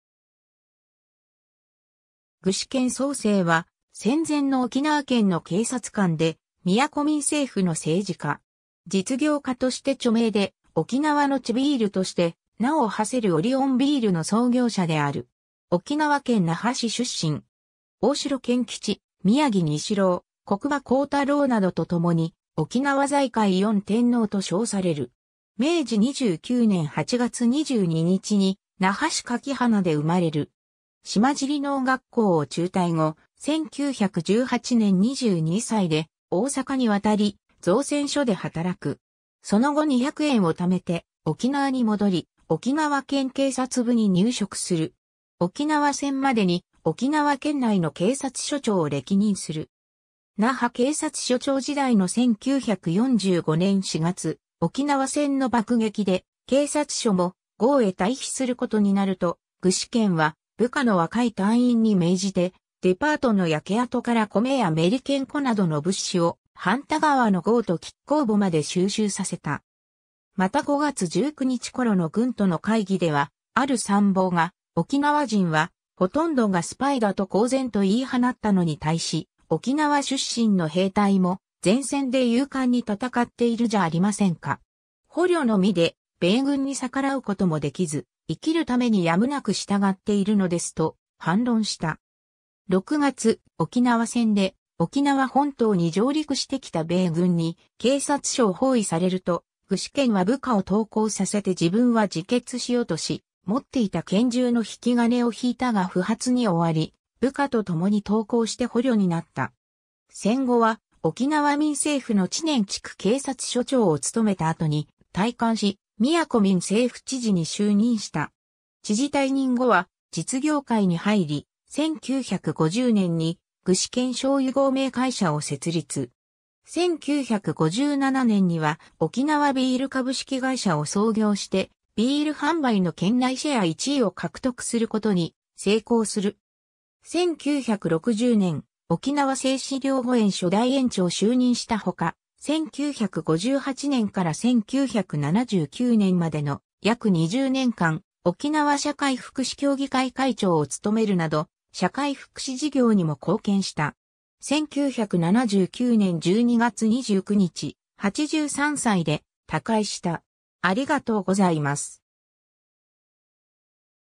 具志堅宗精
各群島・民政府時代の政治家
具志堅 宗精（ぐしけん そうせい、1896年8月22日 - 1979年12月29日）は、戦前の沖縄県の警察官で、宮古民政府の政治家。
実業家として著名で、沖縄の地ビールとして名を馳せるオリオンビールの創業者である。
沖縄県那覇市出身。
大城鎌吉・宮城仁四郎・國場幸太郎などとともに「沖縄財界四天王」と称される。
 明治29年（1896年）8月22日に那覇市垣花で生まれる。
島尻農学校（後の沖縄県立農林学校）を中退後、1918年22歳で大阪に渡り造船所で働く。
その後200円を貯めて沖縄に戻り沖縄県警察部に入職する。
沖縄戦までに沖縄県内の警察署長を歴任する。
 那覇警察署長時代の1945年4月、沖縄戦の爆撃で警察署も壕へ退避することになると、具志堅は部下の若い隊員に命じてデパートの焼け跡から米やメリケン粉などの物資を繁多川の壕と亀甲墓まで収集させた。
また5月19日頃の軍との会議では、ある参謀が「沖縄人はほとんどがスパイだ」と公然と言い放ったのに対し、「沖縄出身の兵隊も、前線で勇敢に戦っているじゃありませんか！捕虜の身で、米軍に逆らうこともできず、生きるためにやむなく従っているのです」と、反論した。
 6月、沖縄戦で沖縄本島に上陸してきた米軍に警察署を包囲されると、具志堅は部下を投降させて自分は自決しようとし、持っていた拳銃の引金を引いたが不発に終わり、部下とともに投降して捕虜になった。
 戦後は沖縄民政府の知念地区警察署長を務めた後に退官し、宮古民政府知事（宮古支庁長）に就任した。
 知事退任後は実業界に入り、1950年に具志堅醤油合名会社（現：株式会社赤マルソウ）を設立。
1957年には沖縄ビール株式会社（現：オリオンビール株式会社）を創業して、ビール販売の県内シェア1位を獲得することに成功する。
 1960年、沖縄整肢療護園（現：沖縄南部療育医療センター）初代園長就任したほか、1958年から1979年までの約20年間、沖縄社会福祉協議会会長を務めるなど社会福祉事業にも貢献した。
 1979年12月29日、83歳で他界した。